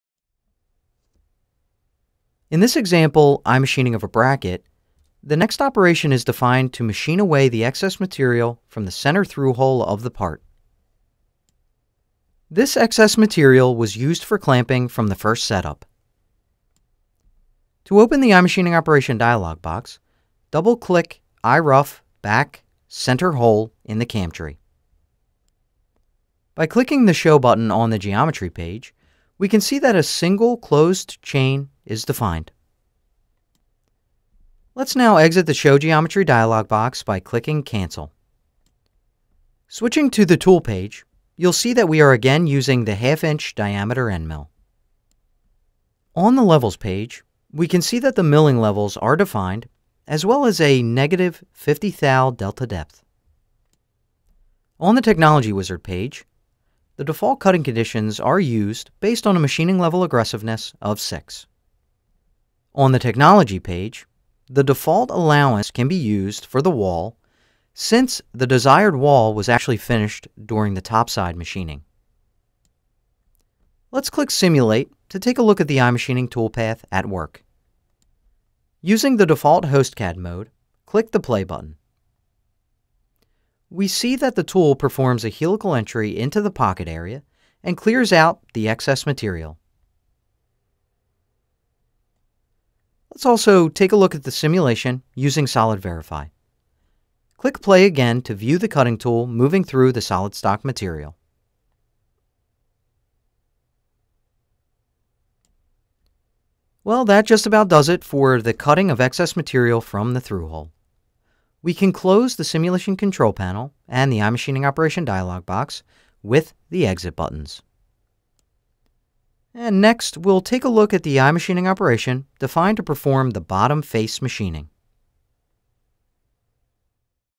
SolidCAM iMachining Getting Started - 2D Example: Cutting of excess material from through hole
This is the seventh recording in a Getting Started series on the iMachining of a Bracket. There are standard 2.5D tool paths (Drilling & Profile) and 3D tool paths (HSR & HSS) to aid in the complete CNC program. This iMachining operation is defined to machine away the excess material from the center through hole of the part. This excess material was used for clamping from the first setup. An end mill of Ø12.5 mm (0.5 in) is used. A single closed chain is defined and the default allowance is used for the wall, since the desired wall was finished during the top side machining.